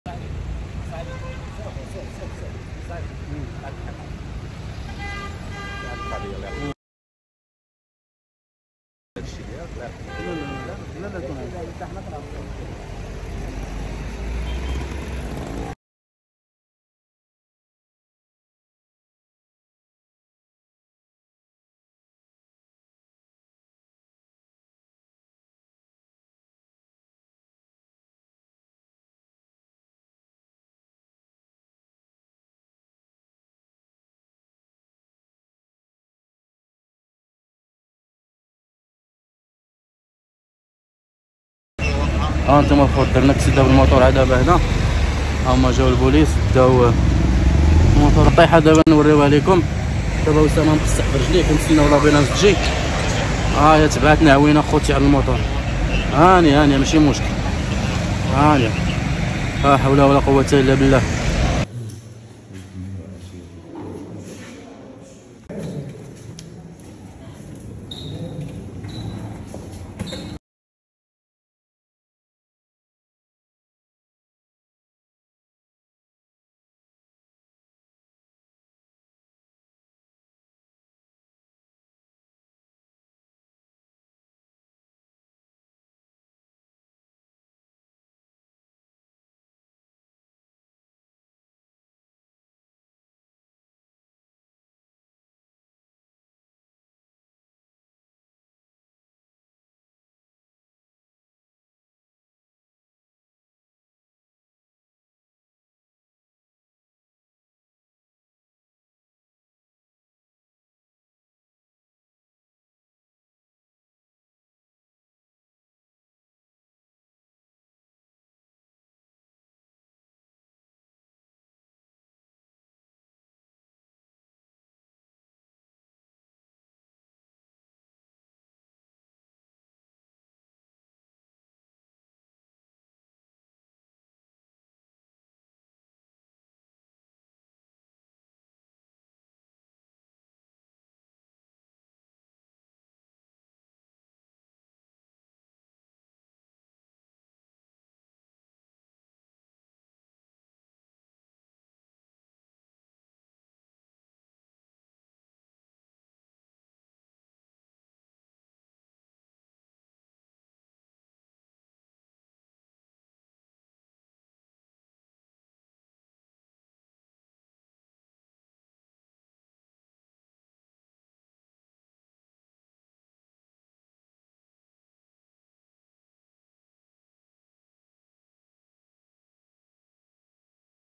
سمايل يو دارو حا.دثة بالموطور 💔😭الحمد الله خرجات طريفة 🙏🏻
لموطور لي عندنا مشا لينا 😭💔